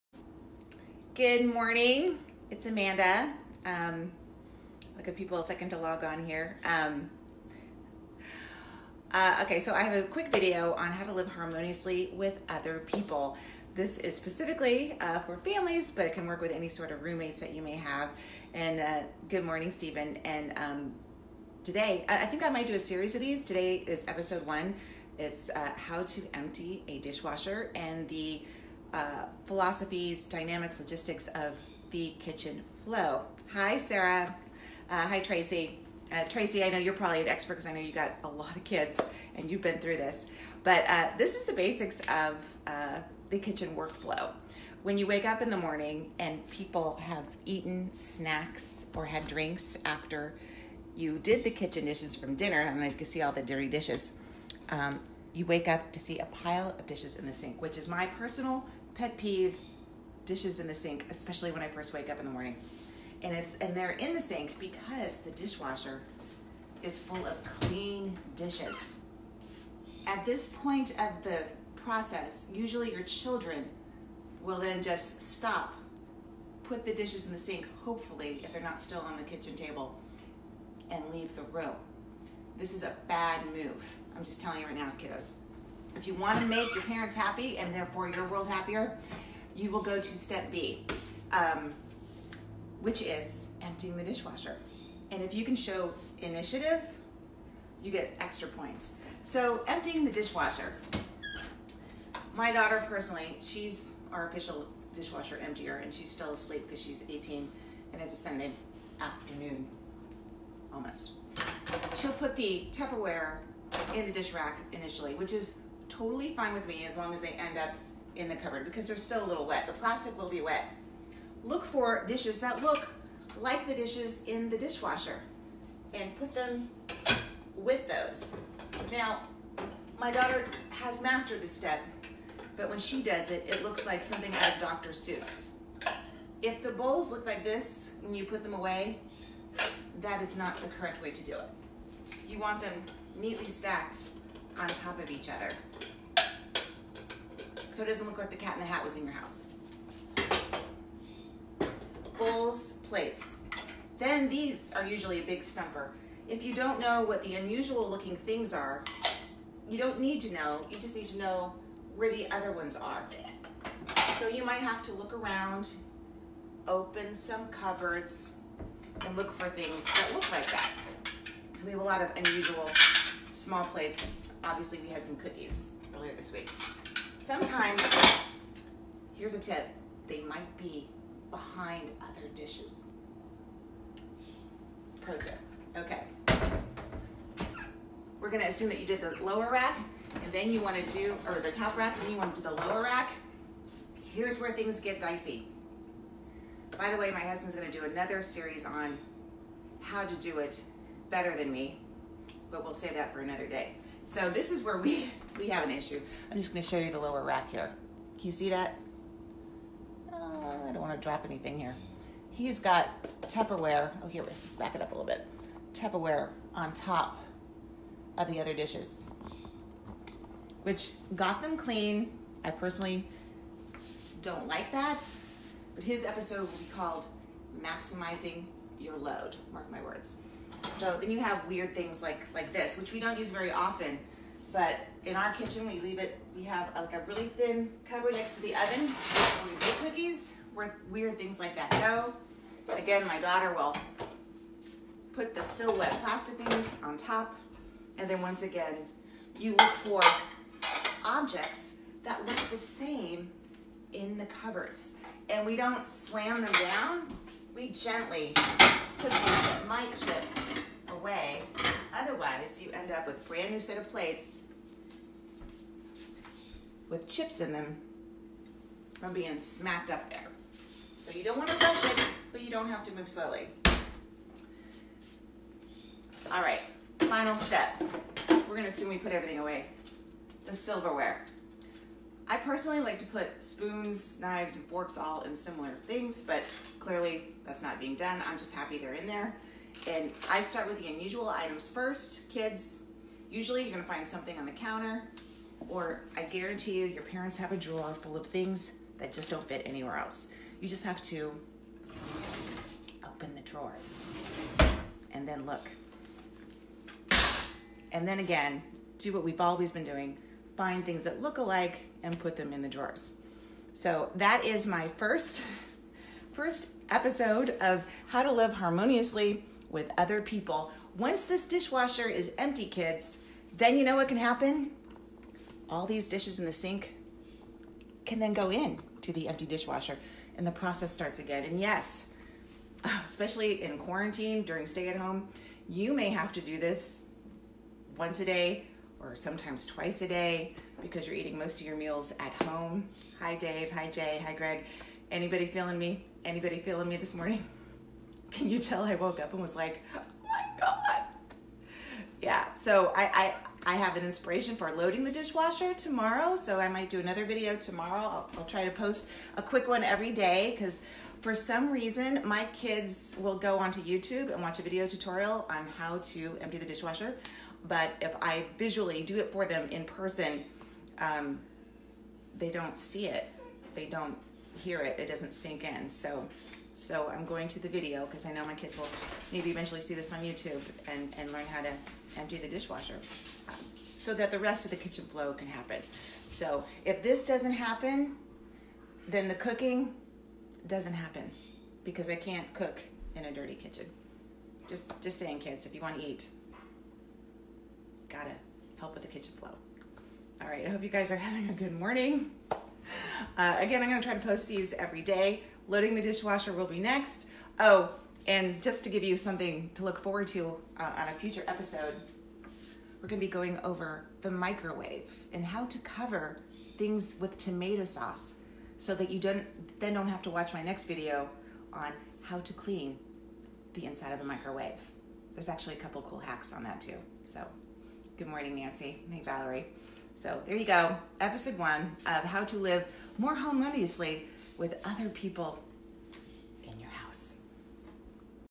Living with QuaranTEENS during the Stay at Home
Amanda gives tips on how to live better together during the Stay at home and really, any day!